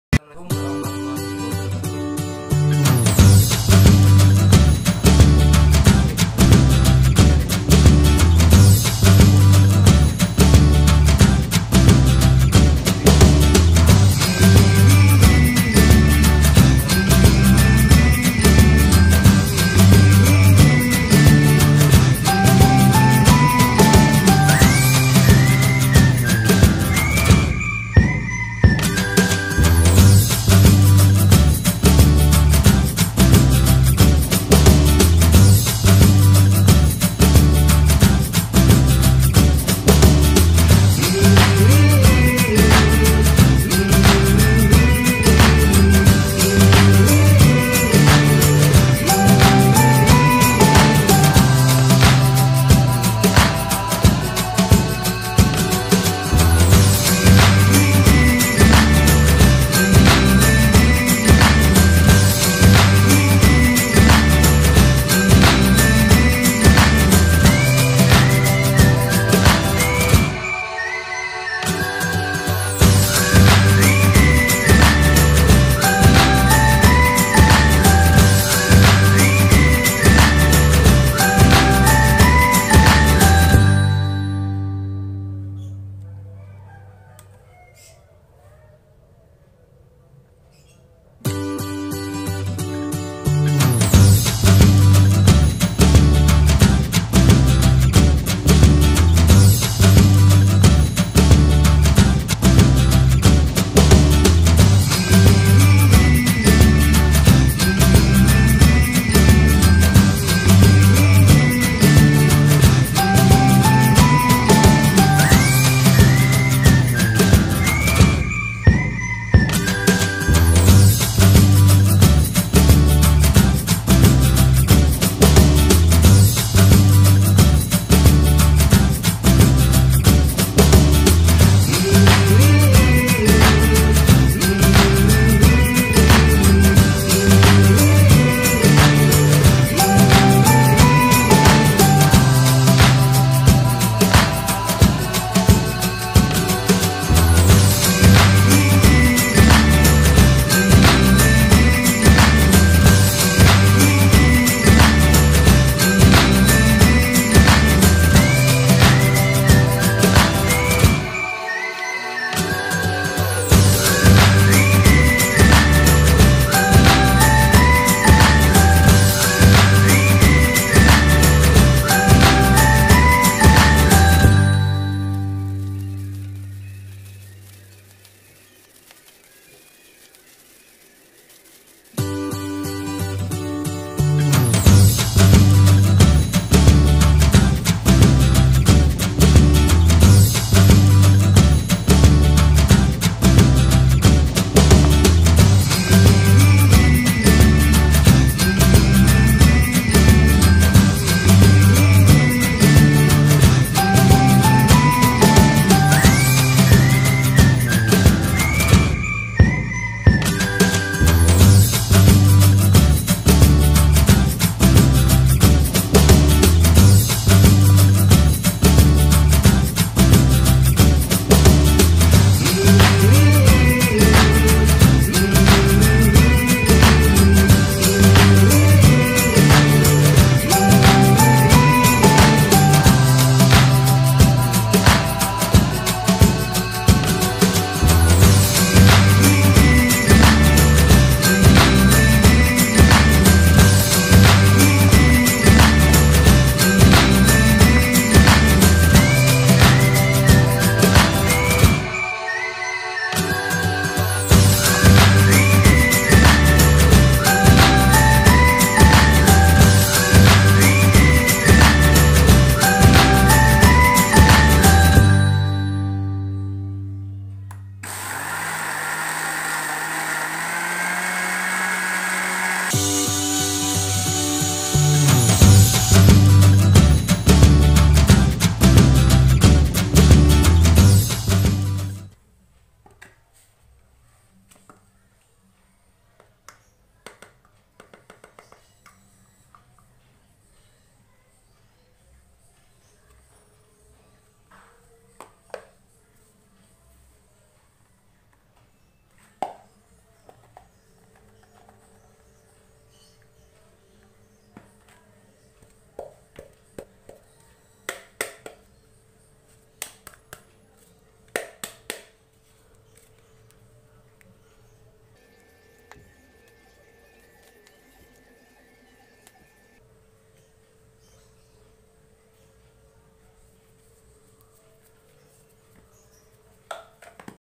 Bakul tempe tutorial: cara membuat tepung terigu berwarna
Dalam bakul tempe tutorial kali ini saya akan memberikan cara membuat tepung terigu berwarna bahan bahannya adalah 1.tepung terigu
              2.pewarna makanan
              3.air secukupnya..
Simak videonya...